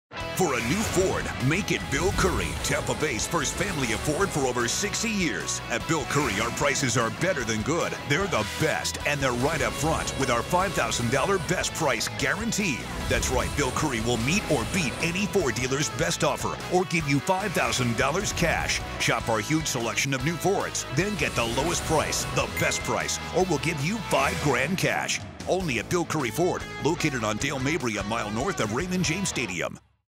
Best Price Guarantee at Bill Currie Ford
Our prices are better than good, they're the best and they are right up front.  They will meet or beat any Ford dealers best offer or give $5,000 cash.